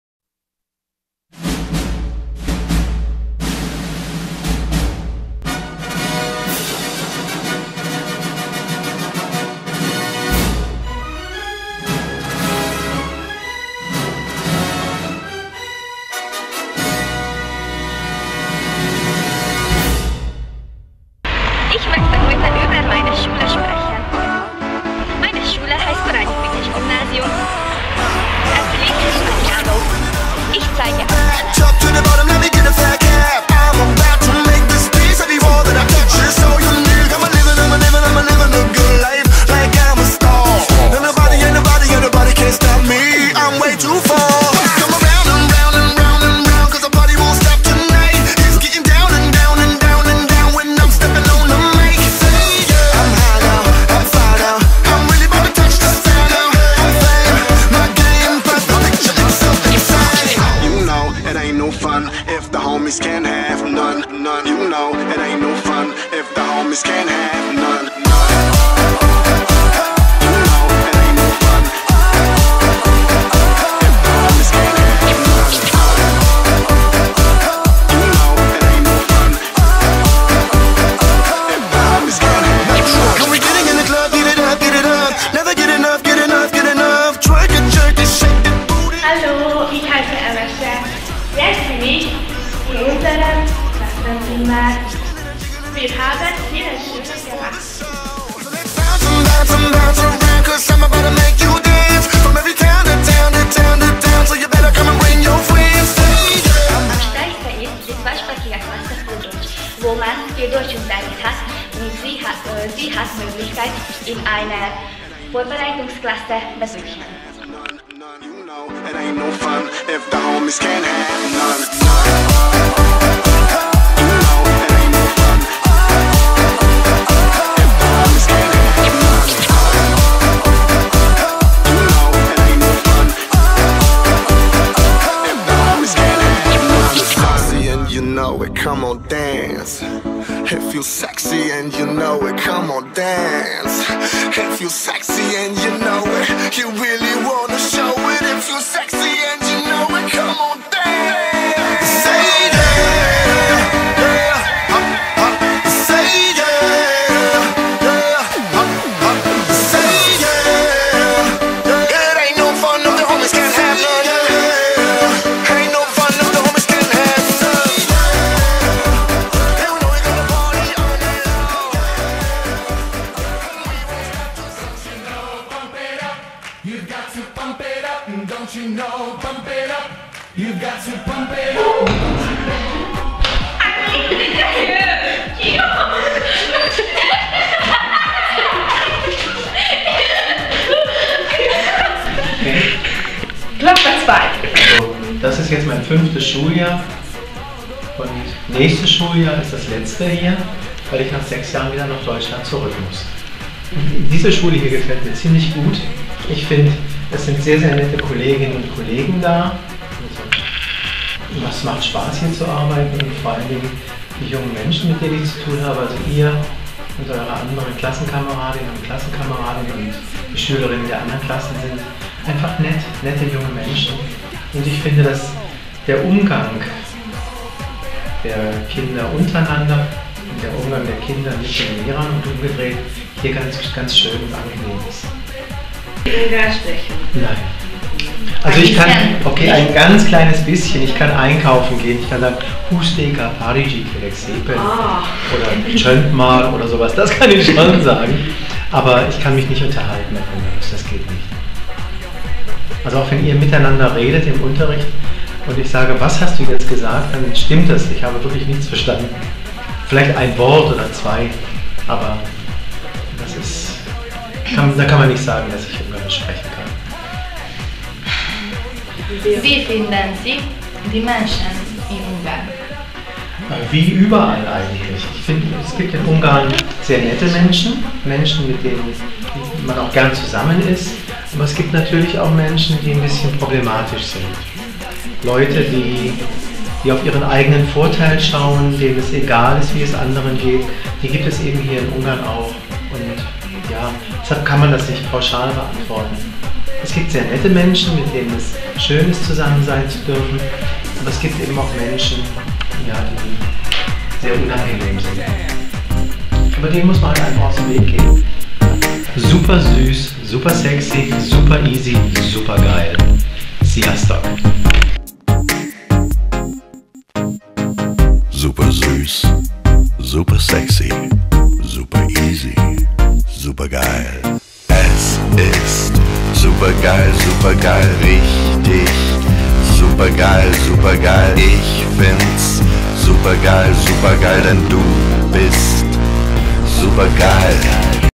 Korányi Frigyes Gimnázium promóciós film /nem hivatalos/
A 9.kny (német kéttan-nyelvű tagozat első osztálya) által készített német nyelvű projektfilm.